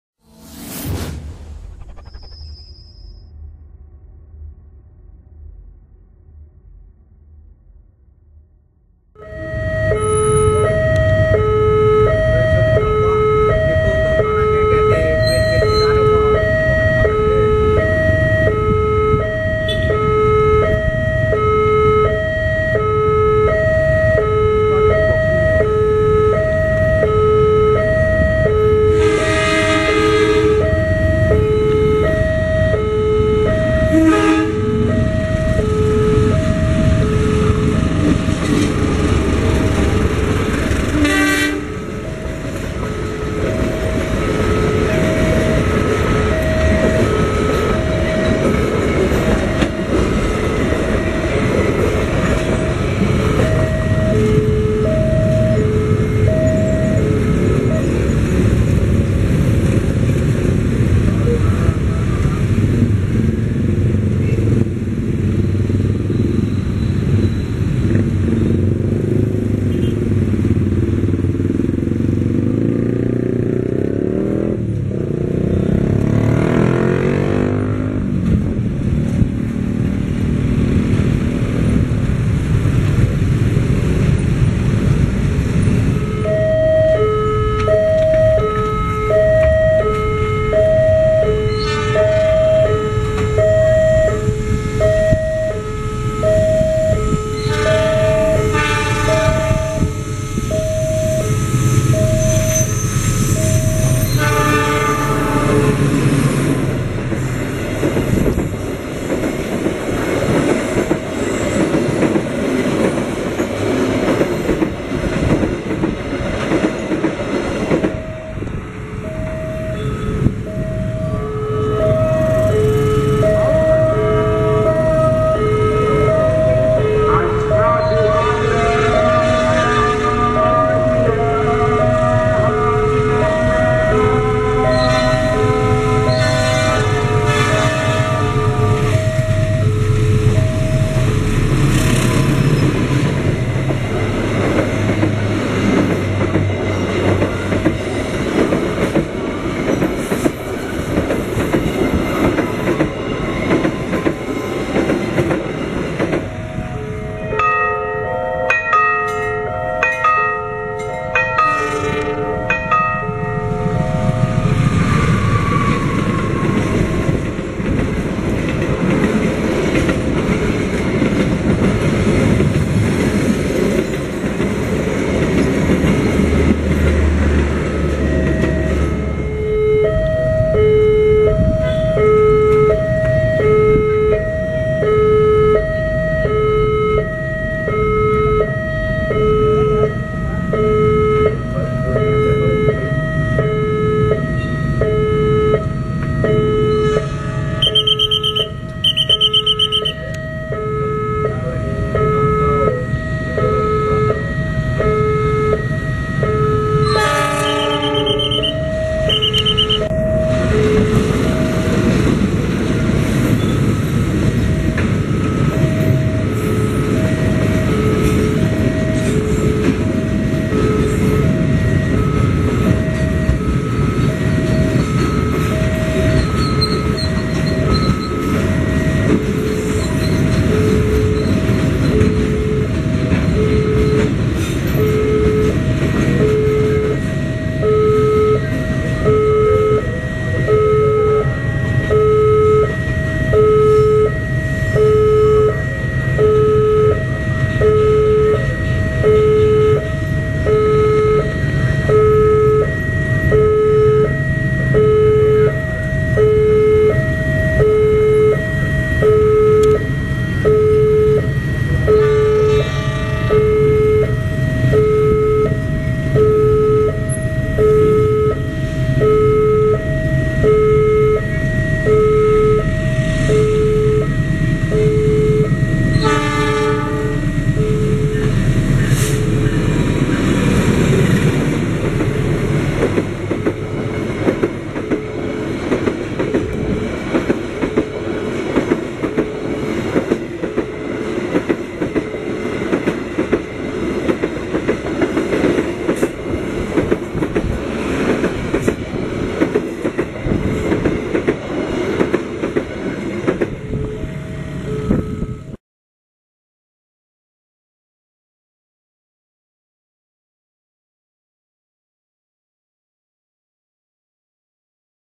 Kesibukan Perlintasan Kereta Api JPL 215 di Siang Hari [2/2]
video ini merupakan kelanjutan dari video sebelumnya tentang kesibukan perlintasan kereta api  JPL  215 di siang hari.
Kereta api yang lewat diantaranya adalah :
1. Ka Tegal bahari
2. Ka Kamandaka
3. KA Sembrani Tambahan
4. Ka Angkutan BBM
5. Ka Tawangjaya